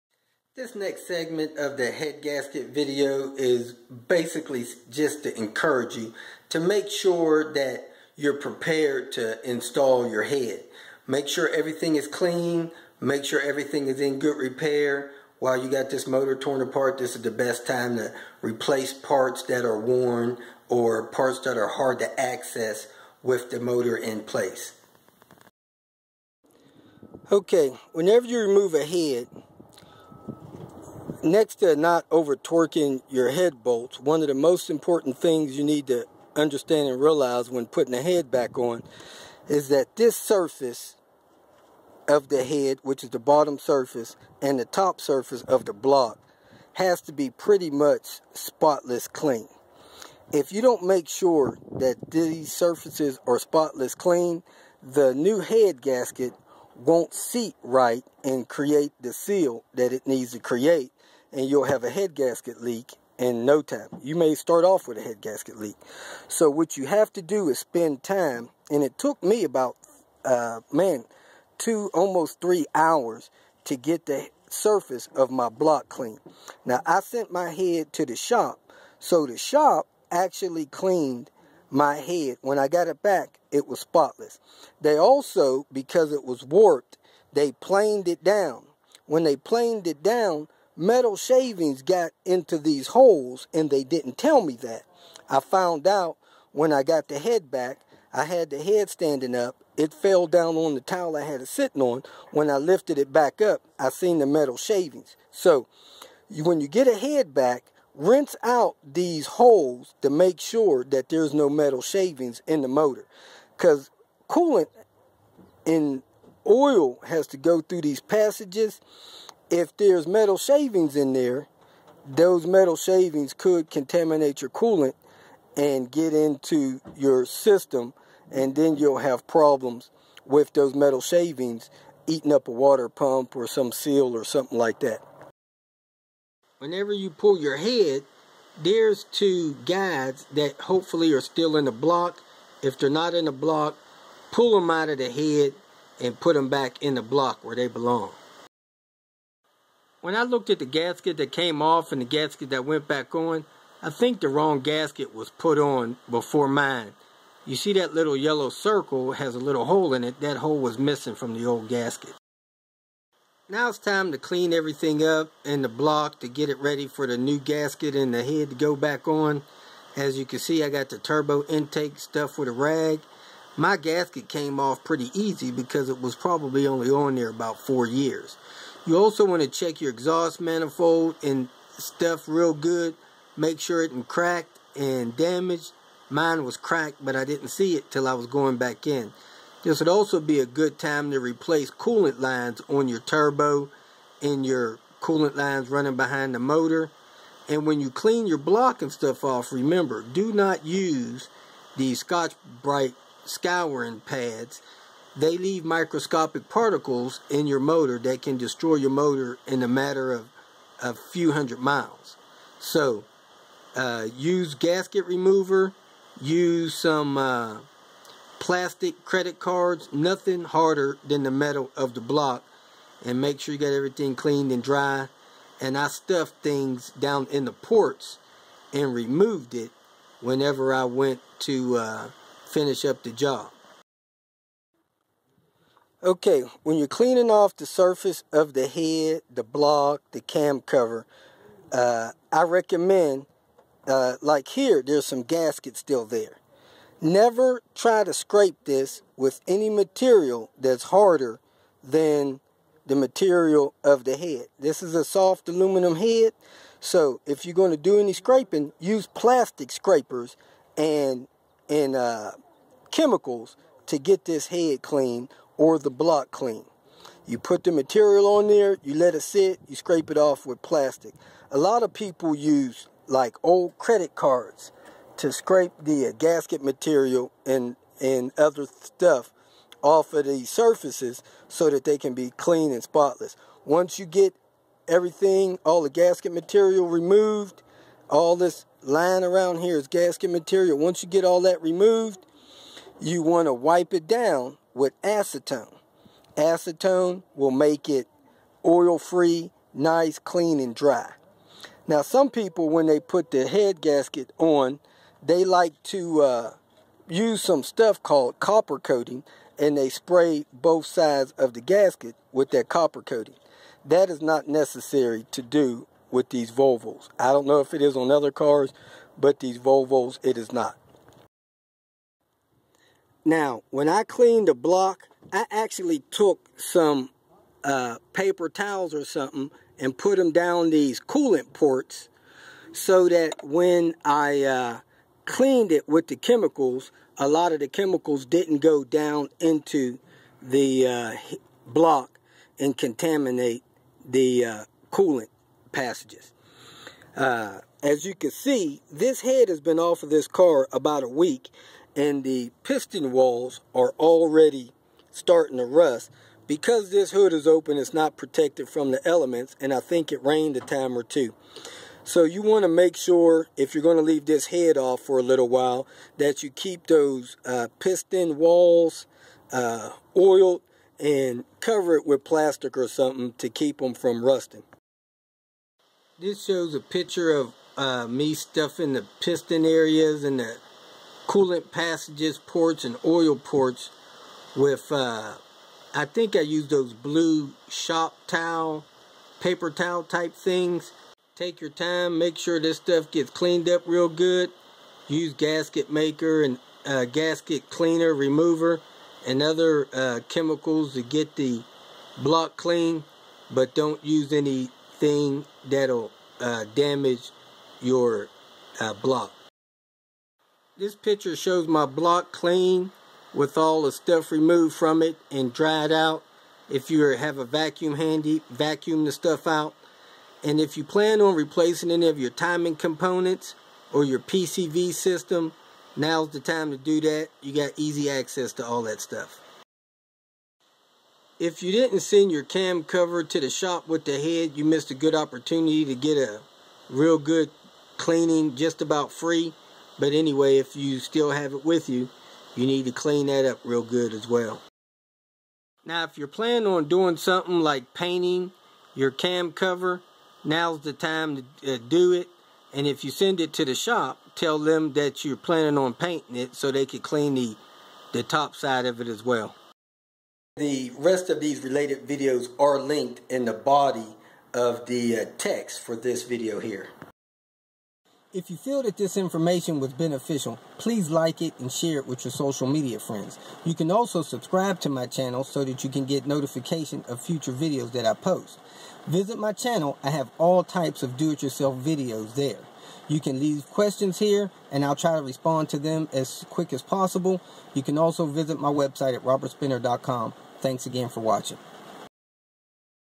Engine cylinder head removed, cleaning & things to do before you install the head information - VOTD
Head cleaning: 2 to 3 hours

Cleaning the block, head, cam cover, and other prep work for installation is very important information. Make sure you have all the gaskets, hoses, and new head bolts ready for the installation of the new or rebuilt head.

I have read that the green scrub pads should NEVER be used to clean the head, block or other parts of the internal motor works parts. They have some very abrasive microscopic metal particles that are often left behind that can DESTROY a motor in days after a rebuild. Just stay away from them.